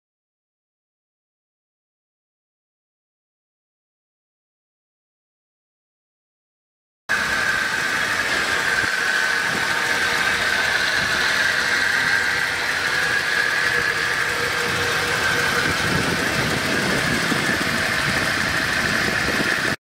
1995 Ford F150 pickup truck for sale | no-reserve Internet auction April 3, 2018
Government Auction
View a detailed listing of this item and place bids

1995 Ford F150 pickup truck
129,157 miles on odometer
4.9L L6 OHV 12V gas engine
Automatic transmission
AC and heat
Cloth interior
Dual fuel tanks
Kumho 235/75R15 tires
Cracked dashboard
Hood damage
Grill cracked
Hole in roof
Unit# 0-159
Kansas title